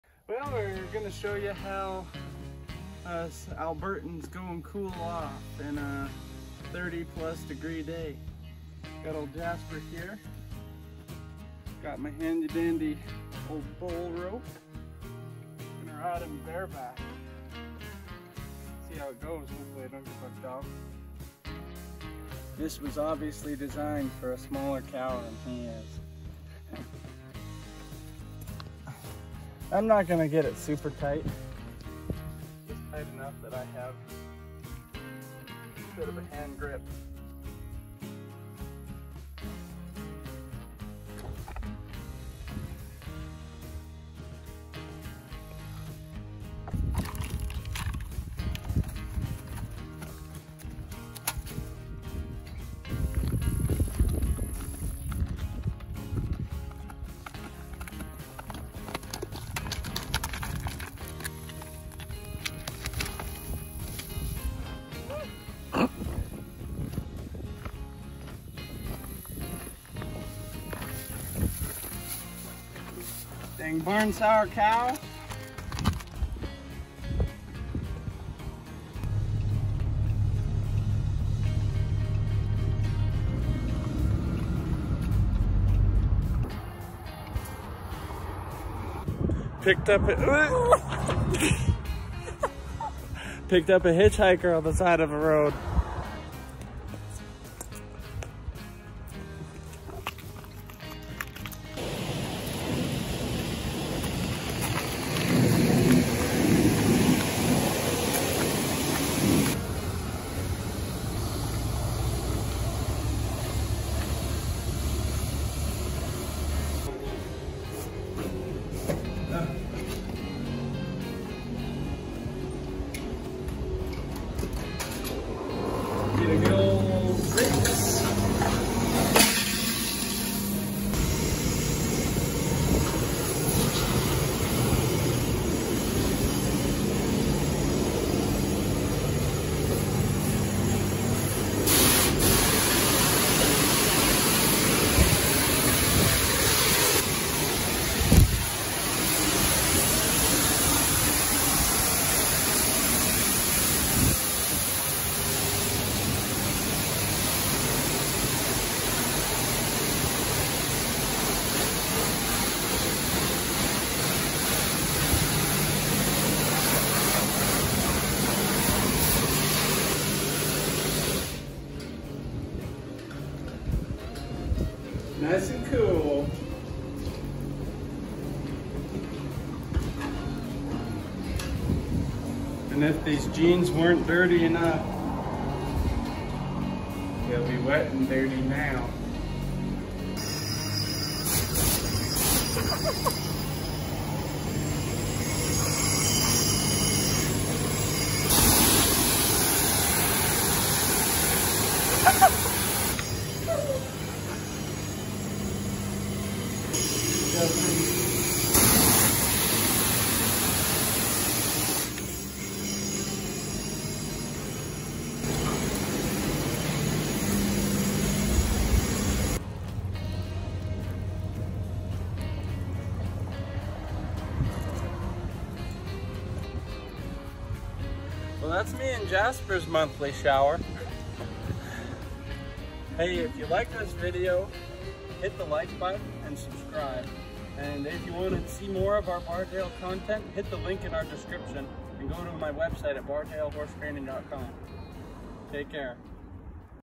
Cow In a CARWASH!
Jasper the Steer is at it again. Joel and Jasper head to the carwash to cool down from the summer heat.

Jasper is a Texas Longhorn cross, 5 year old roping steer. Joel Lybbert trained him at the age of 2 and have been traveling all over entertaining together since. Living life to the fullest is a concept Joel & Jasper grasp to the max. follow us for horse training content, and more fun videos